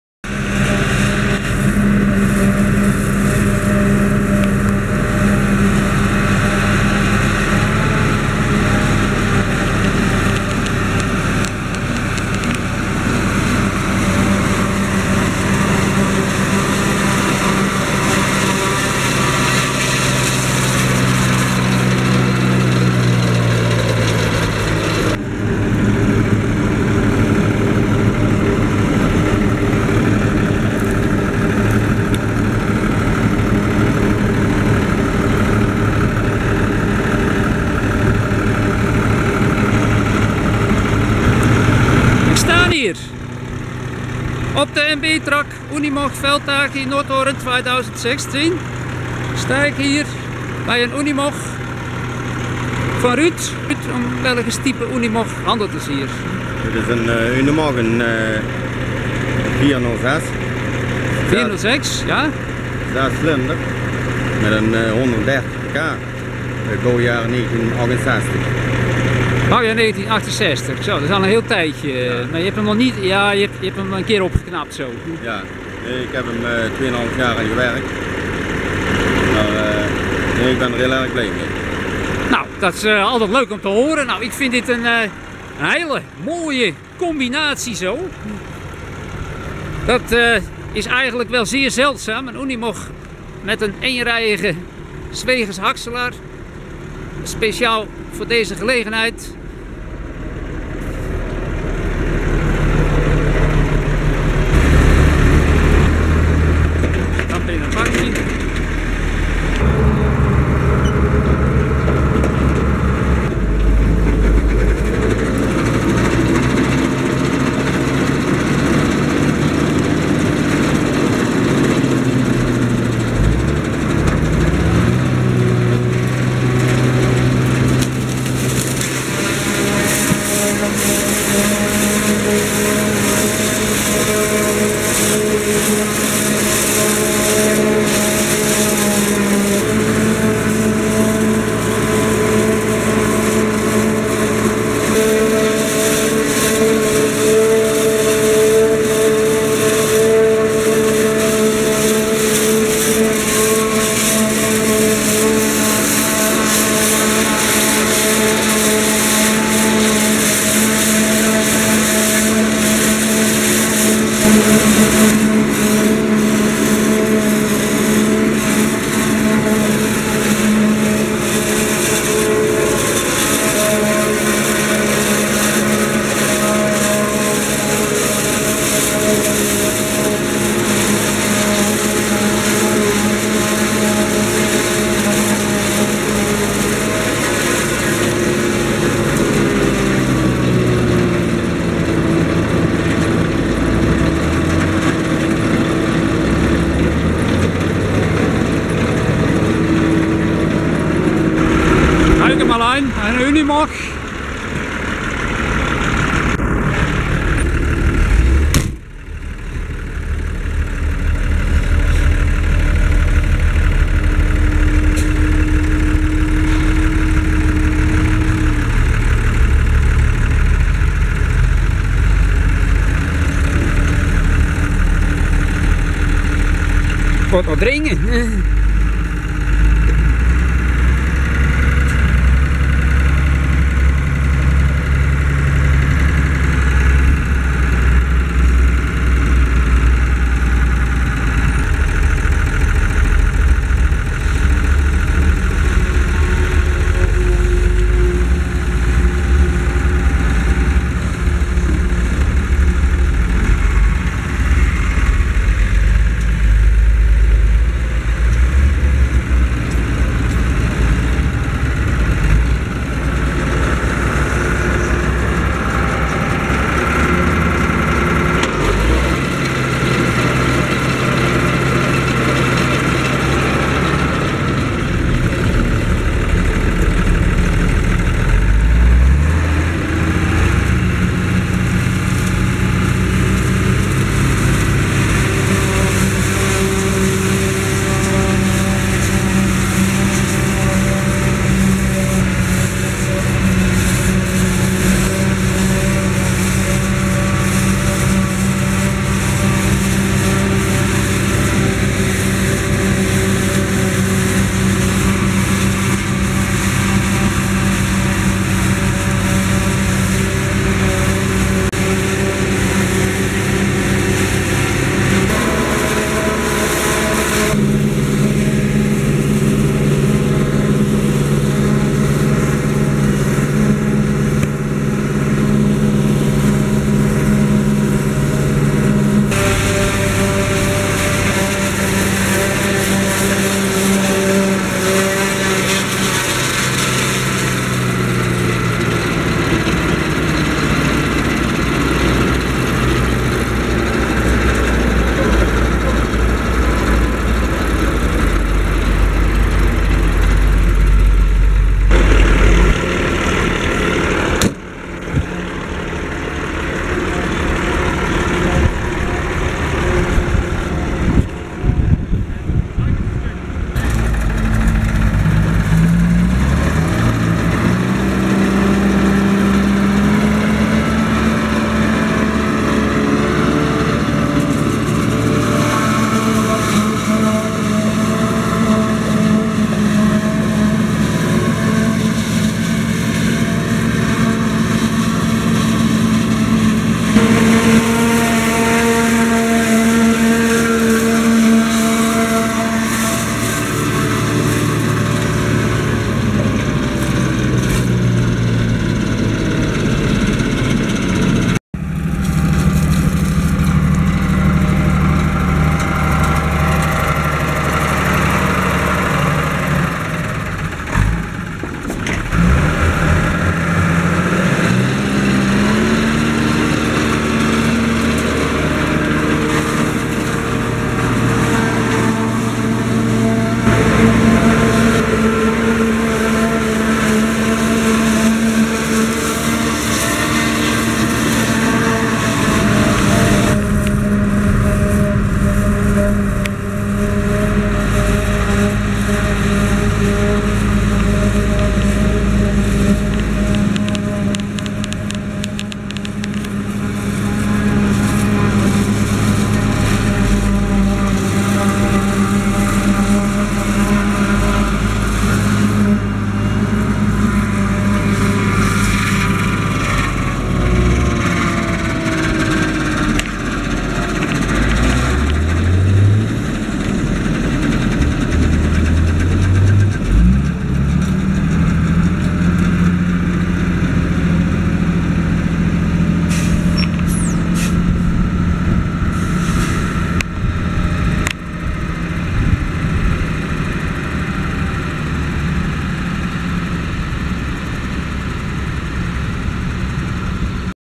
MAISHÄCKSELN mit UNIMOG 406 !! MB-Trac + UNIMOG Treffen Nordhorn 2016.
Einer Unimog 406 geht in einsatz mit einem PZ MH 80 S Maishäcksler !! Mit Cabview !!